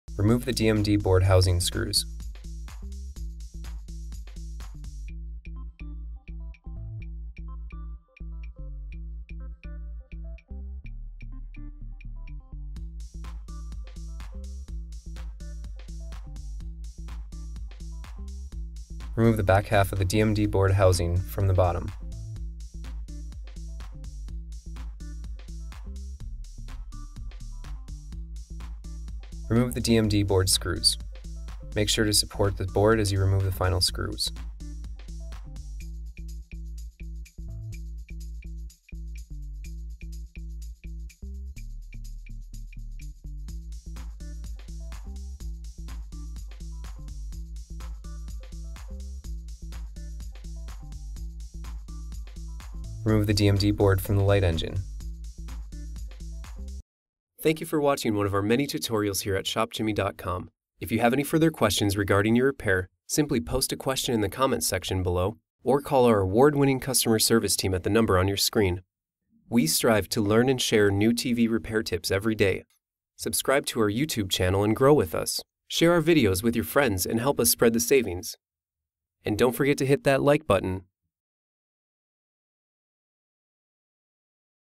Mitsubishi TV - DLP Chip Removal - How to Remove DMD Board from DLP TV - White Black Dots
Mitsubishi DMD Board Removal Help with White Dots Repair

Bad DLP Chips are most commonly found in these TVs:
•  Mitsubishi DLP TV Problems
•  Samsung DLP TV Problems
•  Toshiba DLP TV Problems

Top Common DLP TV Problems for DLP TV Repair:
•  Black & White Dots (Specks, Spots)
•  Stuck Picture (Display)
•  Broken, Stuck Mirrors

What is a DLP Chip?
DLP chips contain millions of microscopic mirrors.  Each mirror can turn towards the lens to reflect light at an exact location on the TV screen, or turn away to help create darker tones.  This technology combined with a color wheel allows for rich tones and dark blacks to appear on a DLP screen.  It is common for a mirror or mirrors within the chip to get stuck in either the on or off position.  When this occurs, fixed white and/or black dots will appear on your screen.  These dots will not move across your screen or blink at any time.  Unfortunately you cannot release the mirrors once they are stuck.  In order to resolve the issue you will need to replace your DLP chip.  Most of the time these stuck mirrors can be viewed by the naked eye.  Lets take a look at a few DLP chips of varying quality.
Here we have an undamaged DLP chip.  It has a perfect reflection. And does not display any dots on the screen.  This is what a damaged DLP chip typically looks like.
Upon close inspection you can see little specks of reflection.  These are are mirrors stuck in the ON position.  In most cases, not that many of the mirrors will be stuck.  Should your DLP screen be plagued with little white specks, your DLP chip will look something like this piece of sparkly art.  It looks like pretty much all of the mirrors are stuck in various positions.  Now that we've explained both symptom and cause, let's go find that DLP chip and replace it!

Part Location:
The DLP chip is located within the light engine or optical engine.

Materials Needed:
To be best prepared to replace the DLP chip inside of your TV. You will need the following materials:
•  A pair of latex gloves.
•  Your ShopJimmy.com replacement DLP chip.
•  A pair of needle-nose pliers.
•  Both a flat-head and phillips screwdriver.
•  And a cup to hold all screws, clips, and any other small items.
So let's get to it shall we?

See Transcript for DLP Chip TV Parts Replacement / Installation Instructions.